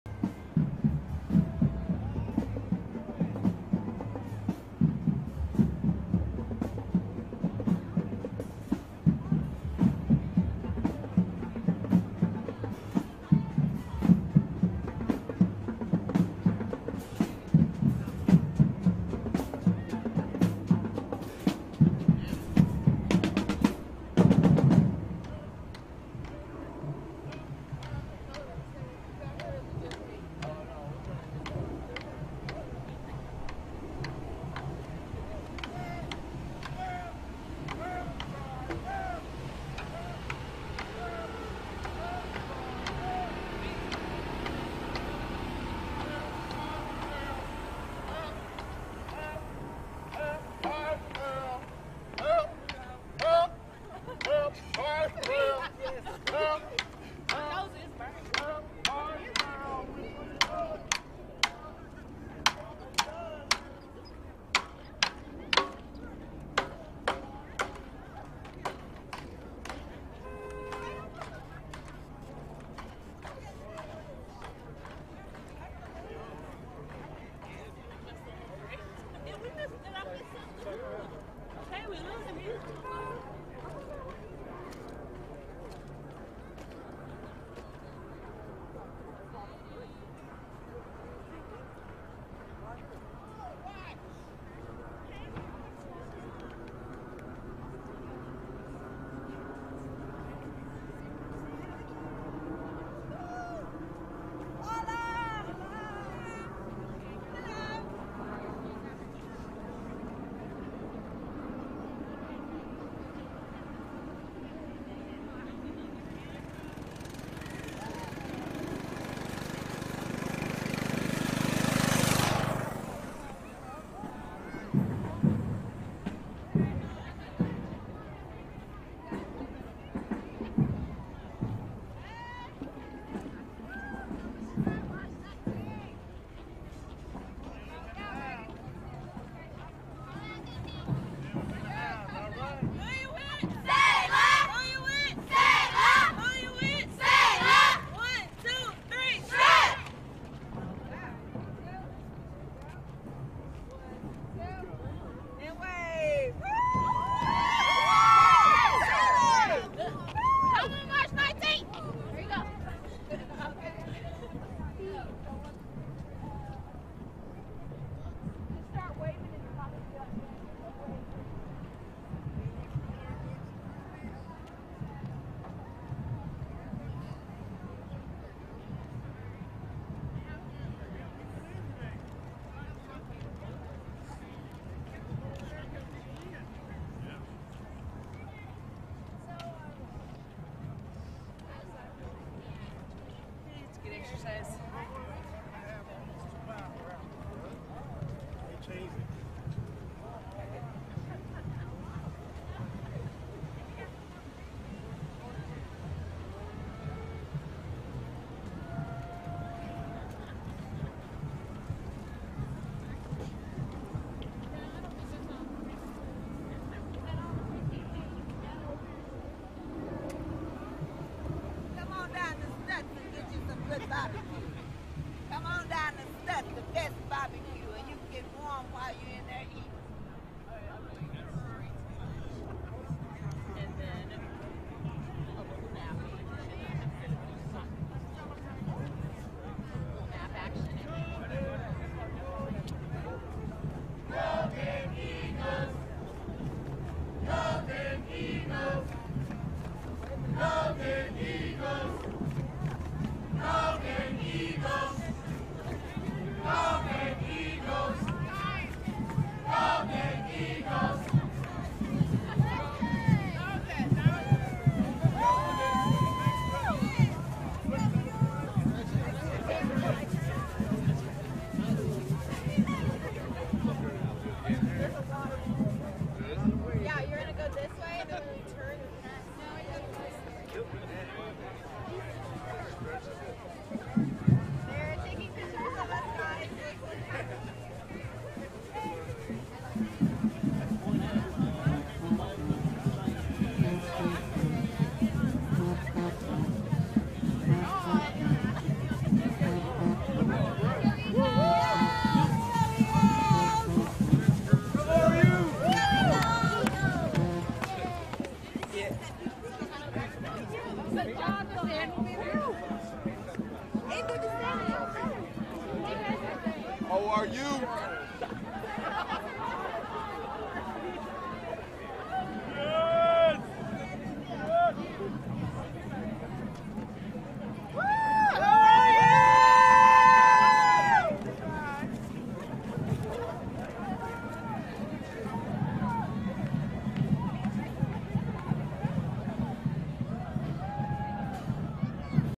MLK Day parade Jan. 18, 2016 in Tulsa
People participate in the MLK Day parade Jan. 18, 2016 in Tulsa.